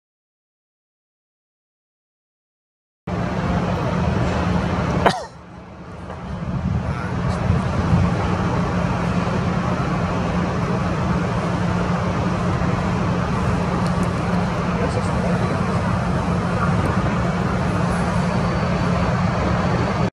Yellow-Breasted Chat - Manhattan, NYC
This Yellow-Breasted Chat has taken up residence at Trinity Church in downtown Manhattan.